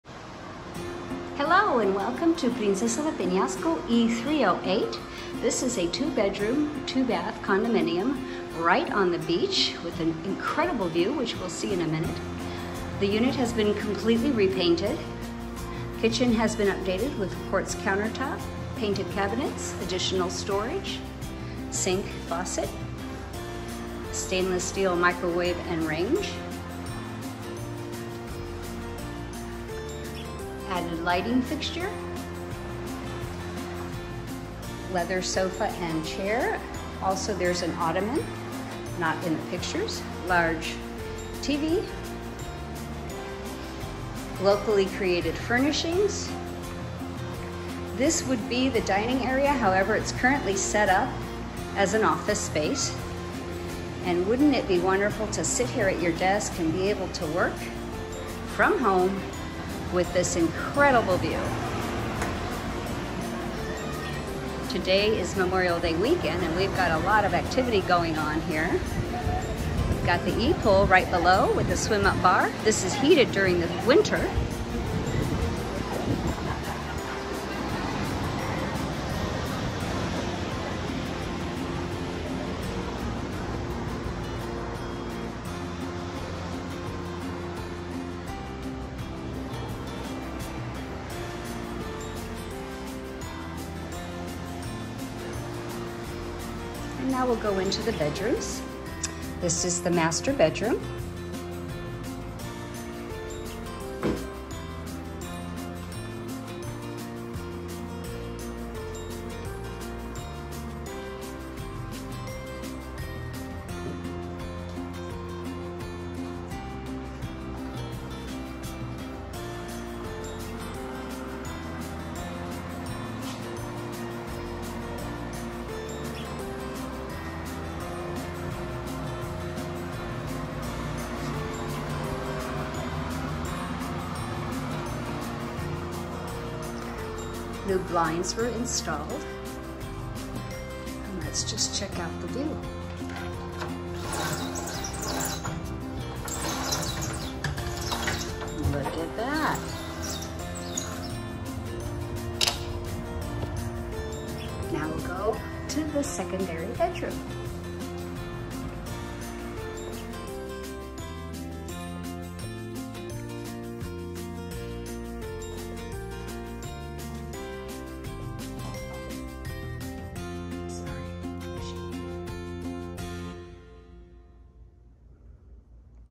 Rocky Point Real Estate | Virtual Tour | Princesa De Peñasco E308 - SOLD
Property Description:
Listed at $347,000 This upgraded 2 bedroom 2 bath condo is located in the highly desired ocean-facing E Building of Princesa de Peñasco and offers spectacular sunset views. Just some of the recent upgrades include interior paint, keyless entry for ease of access, new full-size stainless steel microwave and range, larger quartz countertop, glass kitchen backsplash, additional cabinets in the kitchen and more. These upgrades make this turn-key unit ready for you and your guests to enjoy year round. Princesa offer guarded security, three pools (one of which is heated during the colder months and can be enjoyed year round); swim-up bar; firepit; 16 Palaps including three in the wheelchair accessible area; volleyball court; Bocci Ball court; private owners area that includes a jacuzzi, two BBQ grills, table and fireplace; club house with a gym; and an on-site restaurant (Colin’s Cantina -  Don’t miss Gringo Bingo on Thursdays). This property is held in an Arizona LLC which will provide you with lower closing costs so more money in your pocket at closing.

Offered by:  Angelina Soto

Angelina Sells Rocky Point
Angelina Soto
Mexico Real Estate Rocky Point